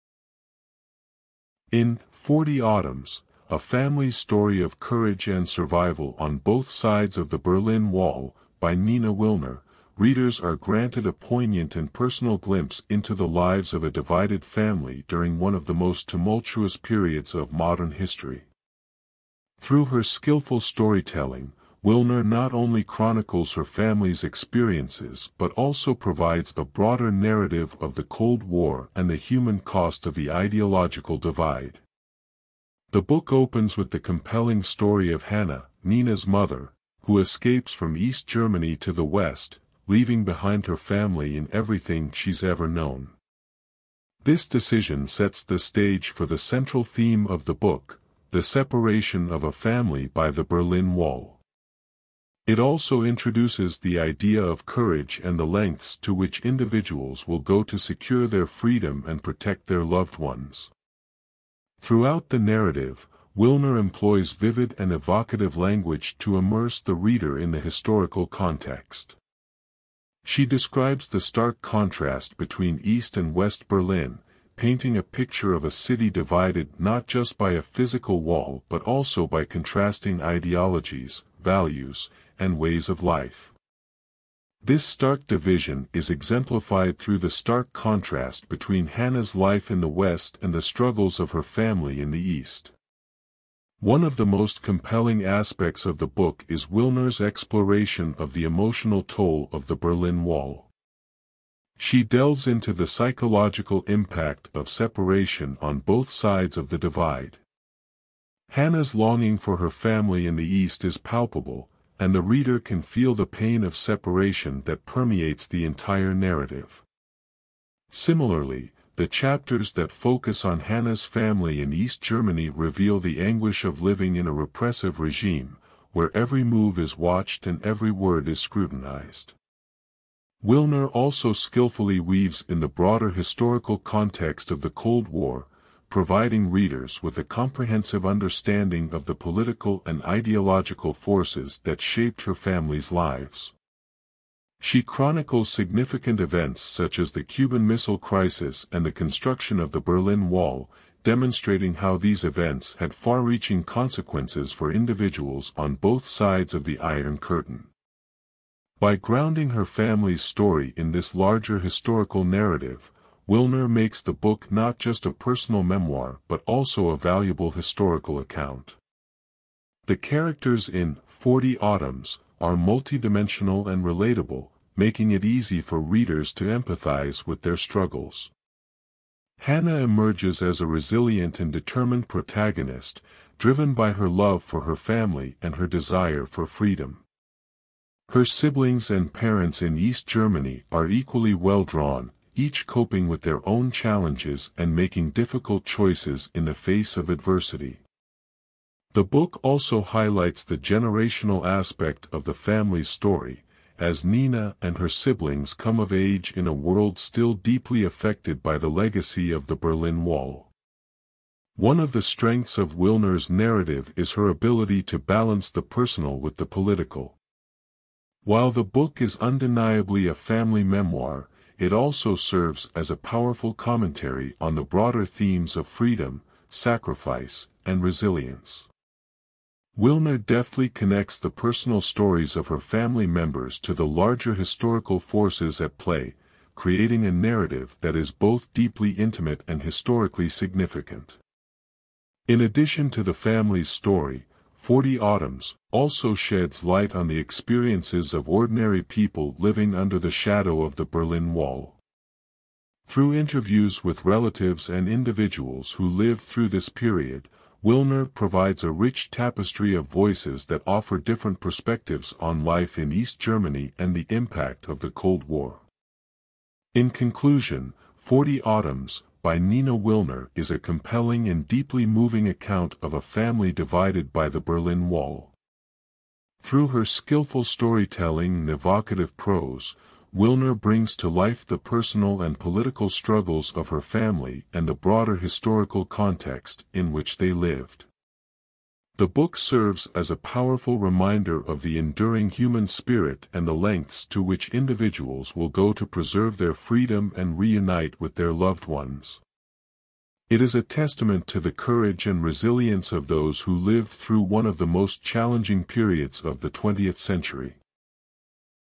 "Forty Autumns" By Nina Willner
In "Forty Autumns: A Family's Story of Courage and Survival on Both Sides of the Berlin Wall" by Nina Willner, readers are granted a poignant and personal glimpse into the lives of a divided family during one of the most tumultuous periods of modern history. Through her skillful storytelling, Willner not only chronicles her family's experiences but also provides a broader narrative of the Cold War and the human cost of the ideological divide.The book opens with the compelling story of Hanna, Nina's mother, who escapes from East Germany to the West, leaving behind her family and everything she's ever known. This decision sets the stage for the central theme of the book – the separation of a family by the Berlin Wall. It also introduces the idea of courage and the lengths to which individuals will go to secure their freedom and protect their loved ones.Throughout the narrative, Willner employs vivid and evocative language to immerse the reader in the historical context. She describes the stark contrast between East and West Berlin, painting a picture of a city divided not just by a physical wall but also by contrasting ideologies, values, and ways of life. This stark division is exemplified through the stark contrast between Hanna's life in the West and the struggles of her family in the East.One of the most compelling aspects of the book is Willner's exploration of the emotional toll of the Berlin Wall. She delves into the psychological impact of separation on both sides of the divide. Hanna's longing for her family in the East is palpable, and the reader can feel the pain of separation that permeates the entire narrative. Similarly, the chapters that focus on Hanna's family in East Germany reveal the anguish of living in a repressive regime, where every move is watched and every word is scrutinized.Willner also skillfully weaves in the broader historical context of the Cold War, providing readers with a comprehensive understanding of the political and ideological forces that shaped her family's lives. She chronicles significant events such as the Cuban Missile Crisis and the construction of the Berlin Wall, demonstrating how these events had far-reaching consequences for individuals on both sides of the Iron Curtain. By grounding her family's story in this larger historical narrative, Willner makes the book not just a personal memoir but also a valuable historical account.The characters in "Forty Autumns" are multi-dimensional and relatable, making it easy for readers to empathize with their struggles. Hanna emerges as a resilient and determined protagonist, driven by her love for her family and her desire for freedom. Her siblings and parents in East Germany are equally well-drawn, each coping with their own challenges and making difficult choices in the face of adversity. The book also highlights the generational aspect of the family's story, as Nina and her siblings come of age in a world still deeply affected by the legacy of the Berlin Wall.One of the strengths of Willner's narrative is her ability to balance the personal with the political. While the book is undeniably a family memoir, it also serves as a powerful commentary on the broader themes of freedom, sacrifice, and resilience. Willner deftly connects the personal stories of her family members to the larger historical forces at play, creating a narrative that is both deeply intimate and historically significant.In addition to the family's story, "Forty Autumns" also sheds light on the experiences of ordinary people living under the shadow of the Berlin Wall. Through interviews with relatives and individuals who lived through this period, Willner provides a rich tapestry of voices that offer different perspectives on life in East Germany and the impact of the Cold War.In conclusion, "Forty Autumns" by Nina Willner is a compelling and deeply moving account of a family divided by the Berlin Wall. Through her skillful storytelling and evocative prose, Willner brings to life the personal and political struggles of her family and the broader historical context in which they lived. The book serves as a powerful reminder of the enduring human spirit and the lengths to which individuals will go to preserve their freedom and reunite with their loved ones. It is a testament to the courage and resilience of those who lived through one of the most challenging periods of the 20th century.